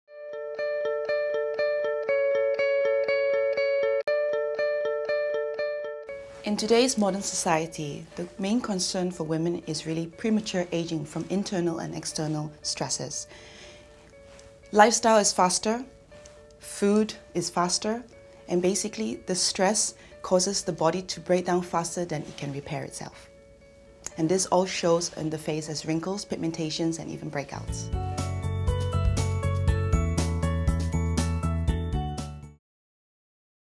Beauté d'Experts n°13 - Web série - The Body Shop
[BEAUTE D'EXPERTS] Stress, pollution, alimentation… De nombreuses agressions que subissent notre peau. Dr Terry nous en dit plus !